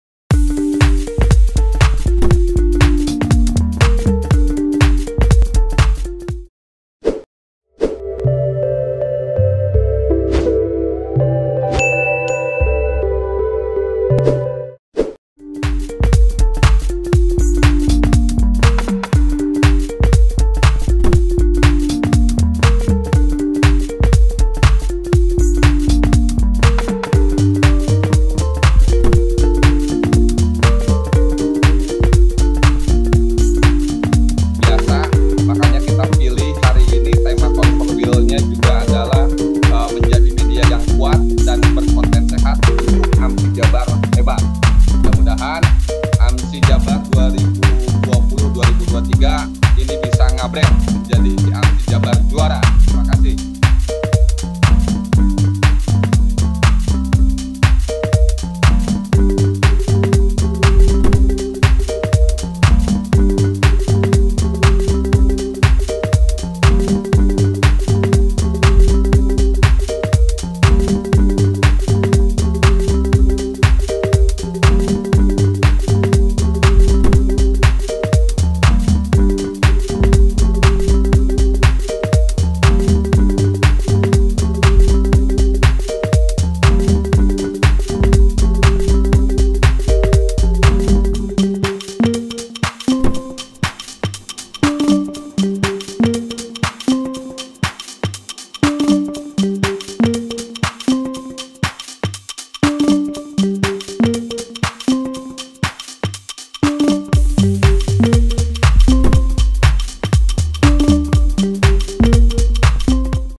REPORTASE - RIANA A. WANGSADIREDJA TERPILIH MENJADI KETUA AMSI JABAR 2020-2023
JABARNEWS | BANDUNG - Asosiasi Media Siber Indonesia (AMSI) Jawa Barat telah melaksanakan Konferensi Wilayah (Konferwil) ke-2, Senin (14/12/2010).

Mengingat masih dalam masa pandemi Covid-19, kegiatan Konferwil ini dilaksanakan secara daring. Dan dihadiri belasan pimpinan media massa online anggota AMSI Jabar serta pengurus AMSI Pusat.